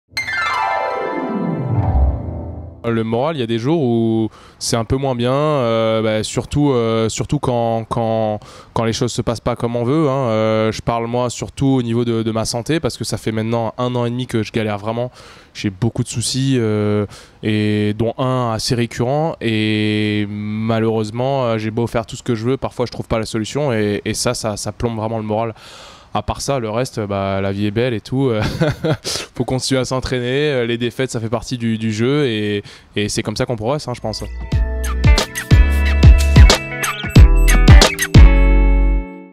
Interview de Gianni MINA
Gianni MINA, d'origine guadeloupéenne jouera en 16ème de finale contre Christian HARRISON (USA)

Crédits : Olivier Jacquemin